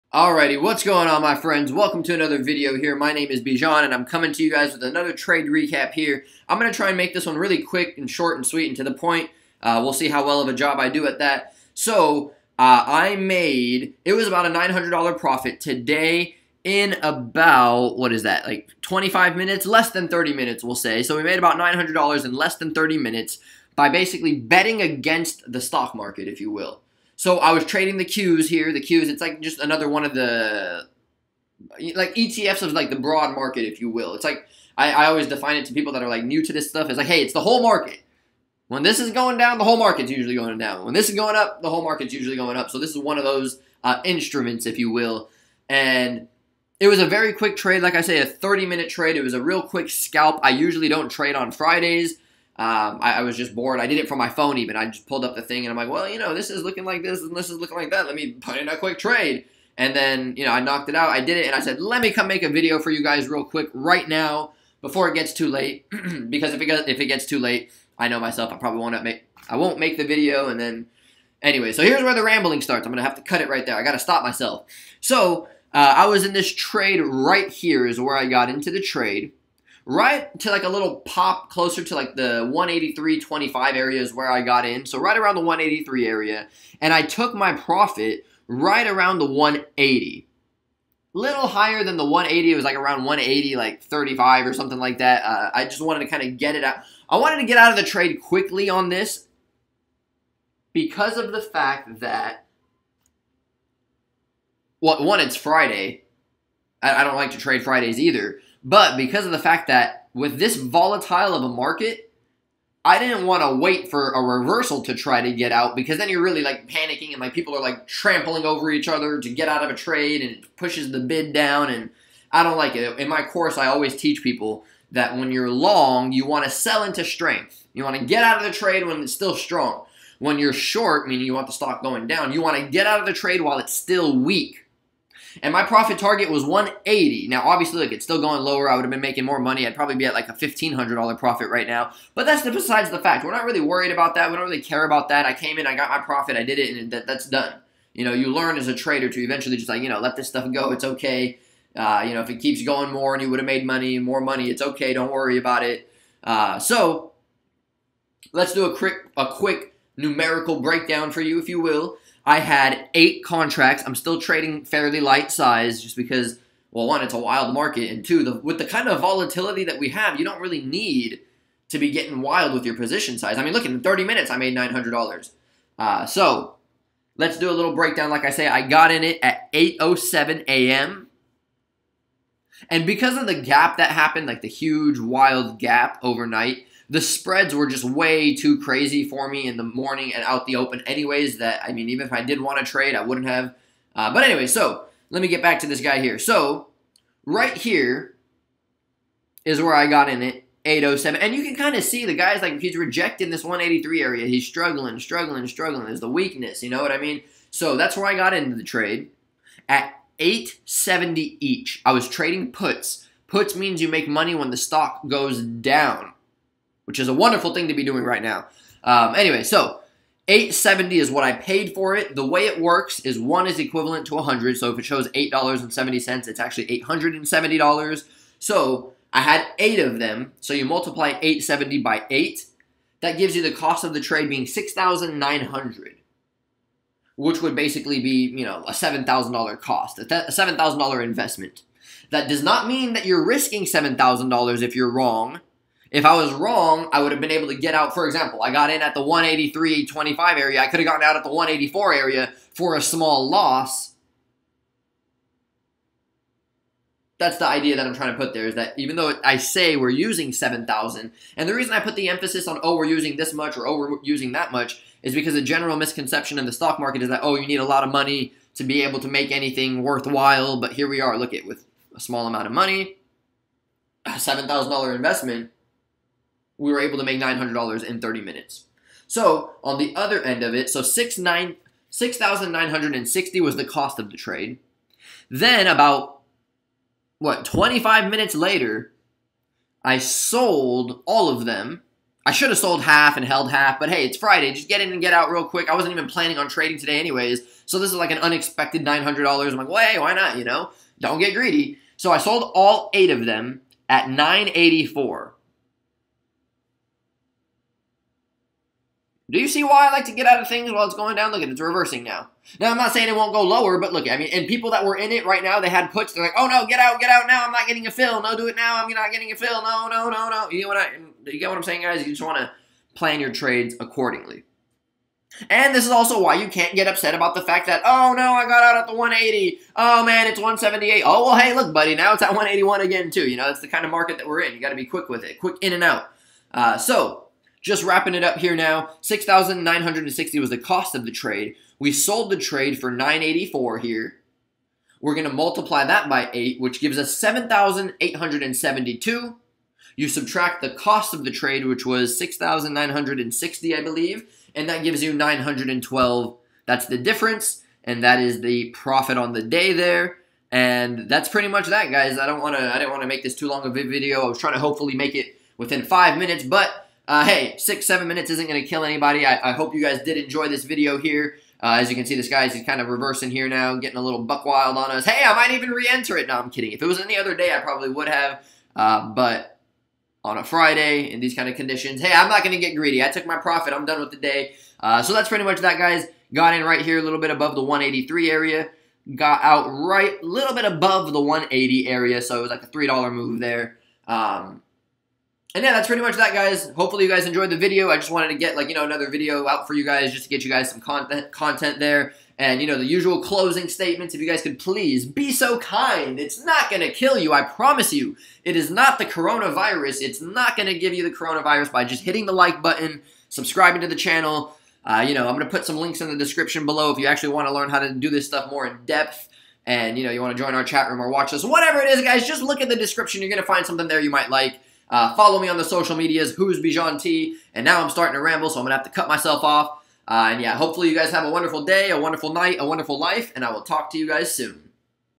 Betting Against the Stock Market Made me $900 in 30 Minutes!?
Just a quick trade recap on a scalp trade that I did to trade PUTS on QQQ. Puts means you can make money with the stock price going down. It was a quick scalp that was only about 30 minutes and the initial investment was less than $7,000. Just always remember that options trading and just trading in general can be risky so please don't just blindly jump into this stuff and never trade with money that you aren't completely willing and able to lose! I hope you guys enjoy the video!